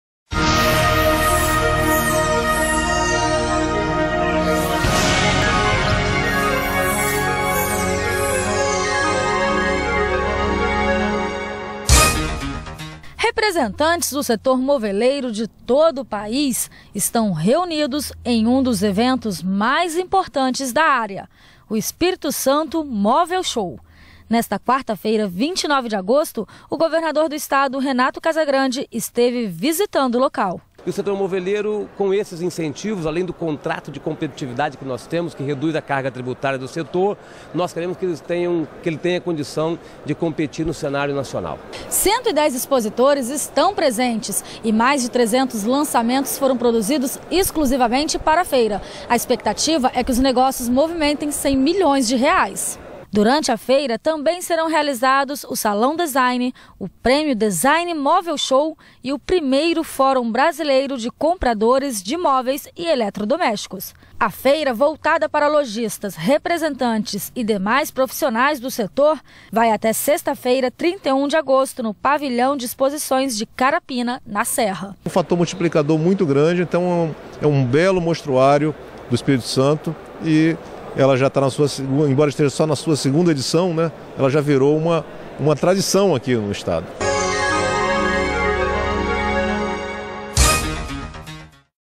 Representantes do setor moveleiro do Brasil se reúnem no Espírito Santo Móvel Show
Mais de 300 lançamentos foram produzidos exclusivamente para a feira. A expectativa é que os negócios movimentem R$ 100 milhões